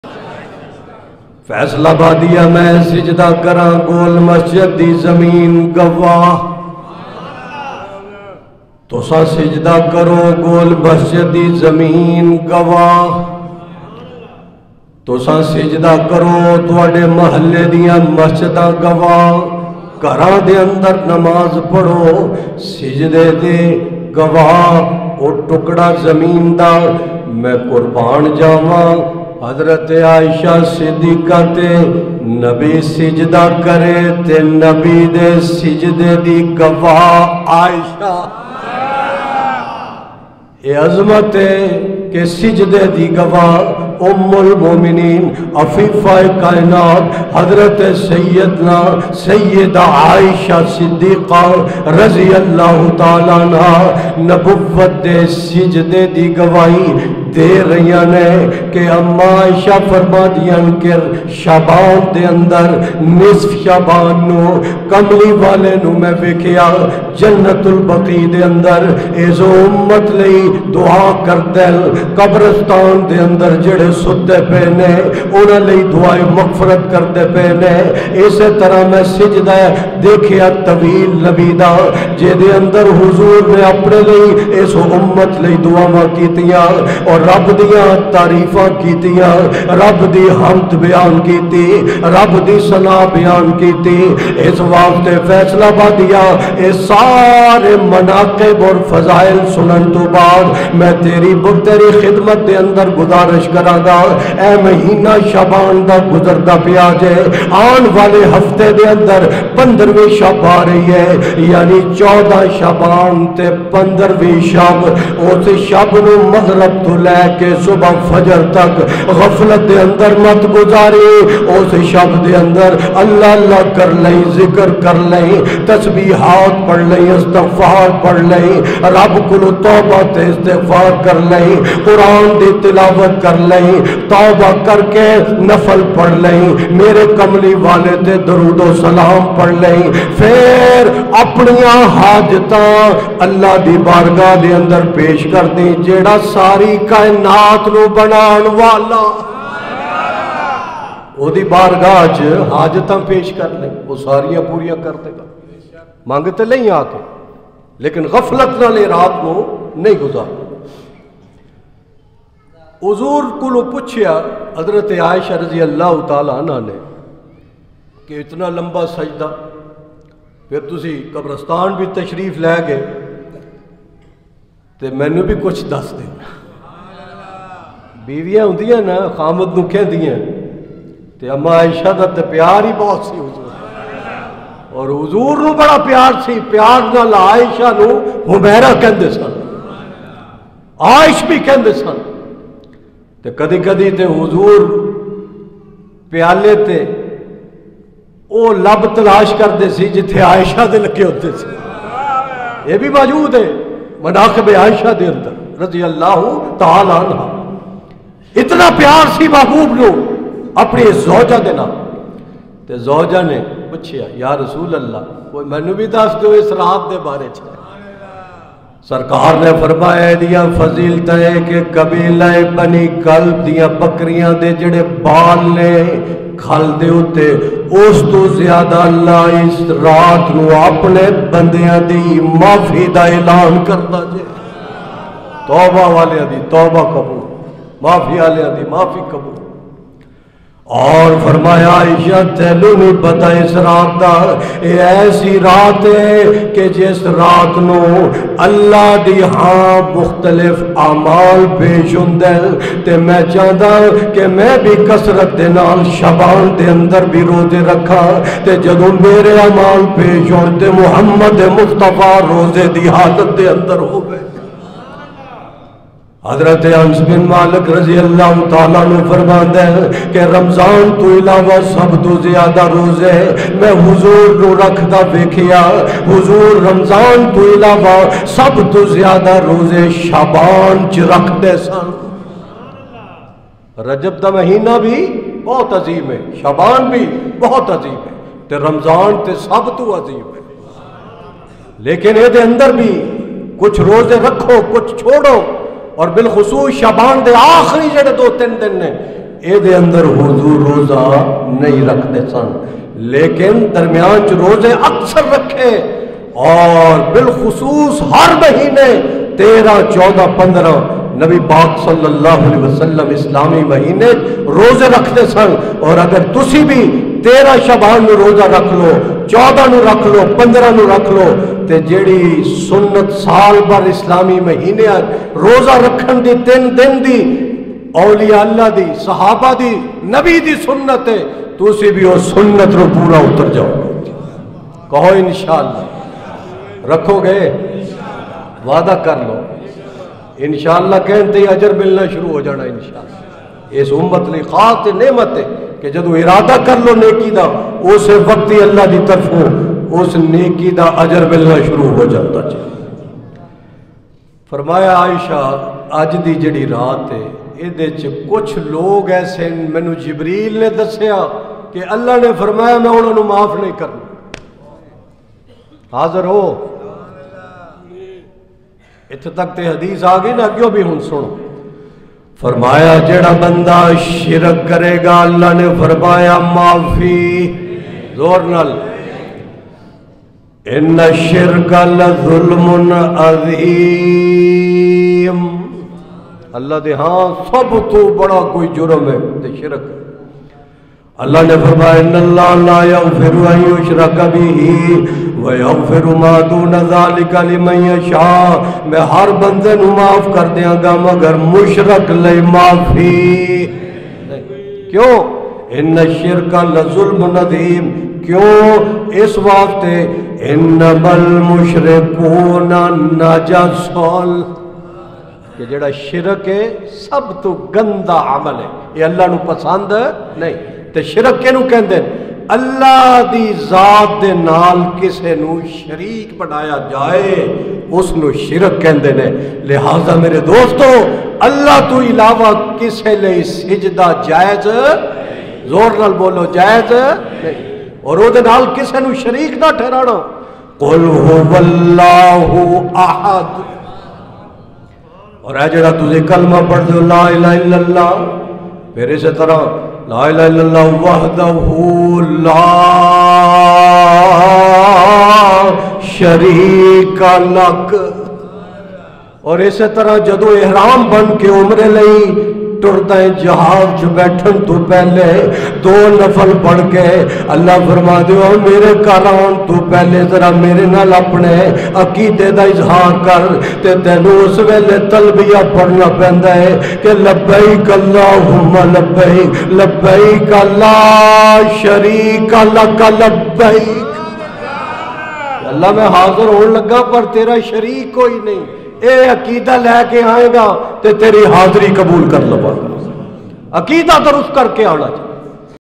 Shab e Baraat | شب برأت | Sahibzada Zahid Mahmood Qasmi | جمعۃ المبارک | Jamia Masjid Goal | TZQ
موضوع : شب برأت

‎بیان: جانشین امام الخطباء حضرت مولانا صاحبزادہ زاہد محمود قاسمی دامت برکاتہم عالیہ
‎‏‎ (چیئرمین مرکزی علما کونسل پاکستان)‏